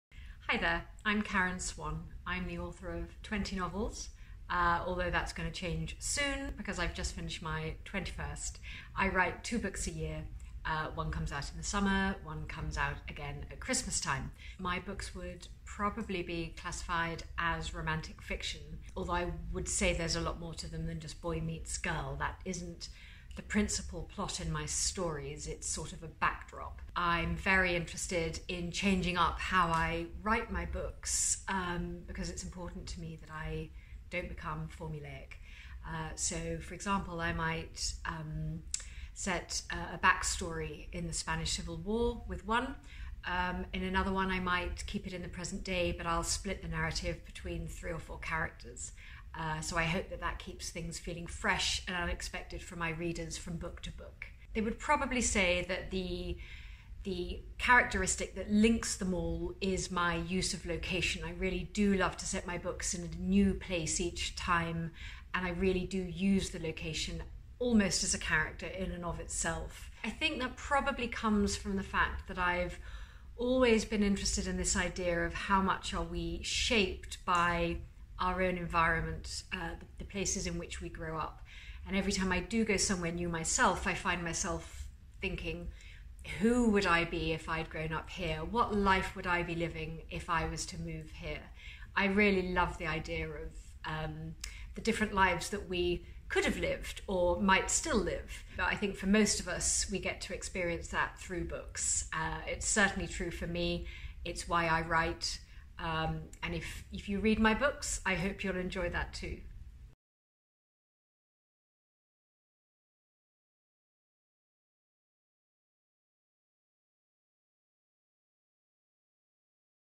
Meet Karen Swan
Meet the bestselling author of Together By Christmas and The Spanish Promise, Karen Swan.